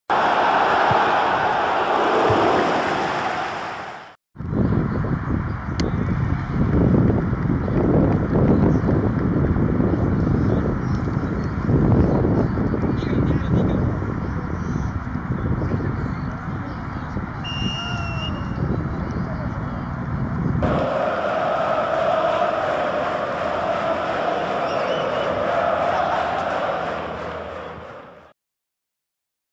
TOCATV: U13 Boys WARRIORS (Goal by Fede)
2021 SPRING SEASON
Sunday, May 23, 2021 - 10:30 AM
MSI Classic League
U13 Boys WARRIORS vs FC Maryland
Julius West MS - 9A (651 Great Falls Rd., Rockville, MD 20850)